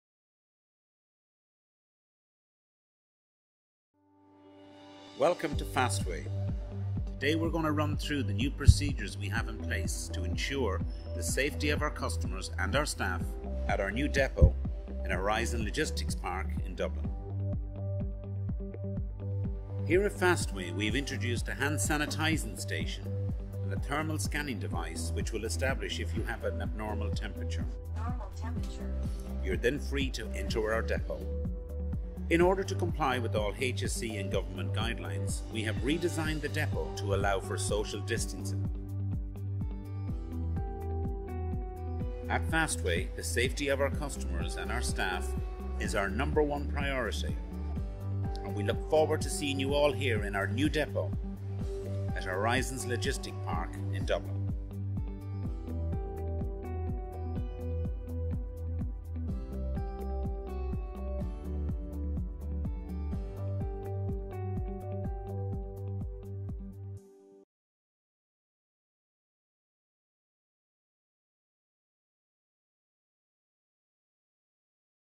Fastway Couriers - DAMOC Thermo Check
Fastway Couriers Ireland have implemented the DAMOC Thermo Check in their new depot in Horizon Logistics Park, helping to keep their staff and customers safe.

Get in touch today and find out how Focal Media can help your business implement new procedures!